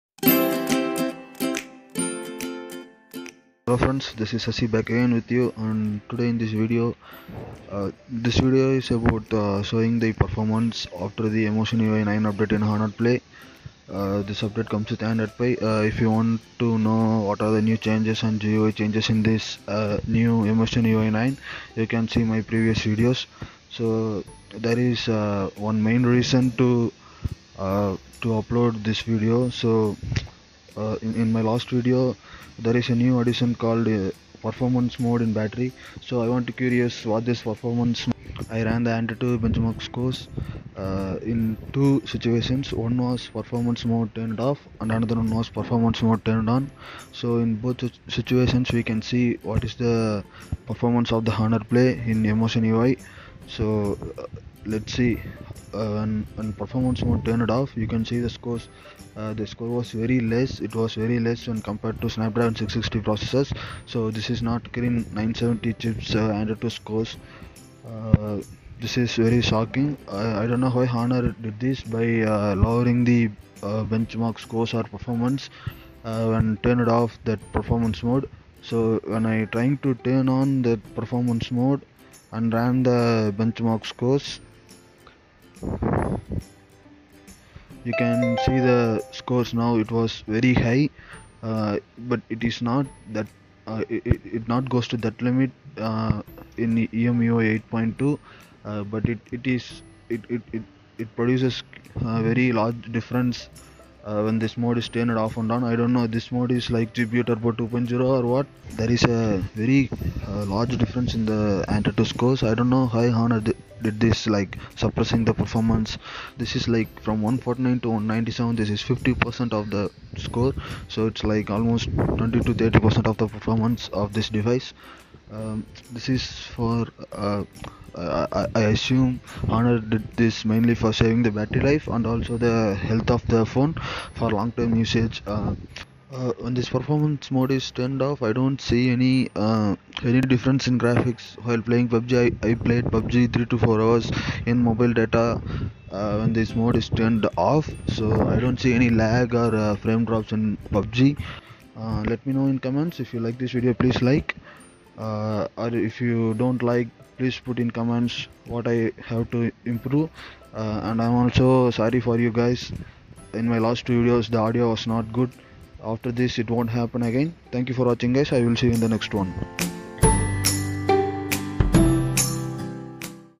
EMUI 9 Benchmark score in Honor Play
Hello friends, In this video, I will show the Antutu benchmark score in Honor Play after Emui 9 update.

There is a new option under battery called Performance mode. I ran Antutu benchmark when this mode on and off and see what the scores.